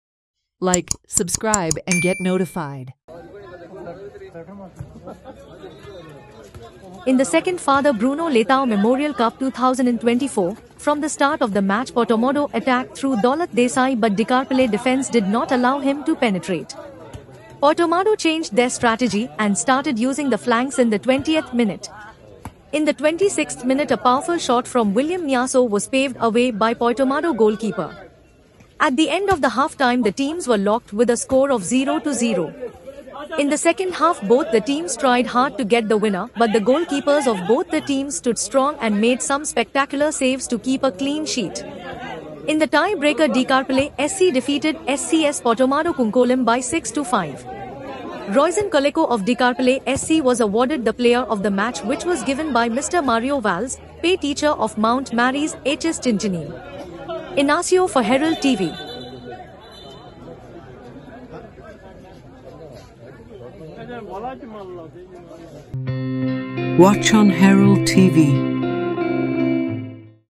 Dicarpale SC beat SCS Poitamado Cuncolim
Dicarpale SC defeated SCS Poitomado Cuncolim 6-5 via tie breaker to enter the semi finals of the  2nd Fr. Bruno Leitao Memorial Cup 2024. The thrilling match was organized by  Union of Chinchinim villagers  and played on Chinchinim Church ground on Saturday.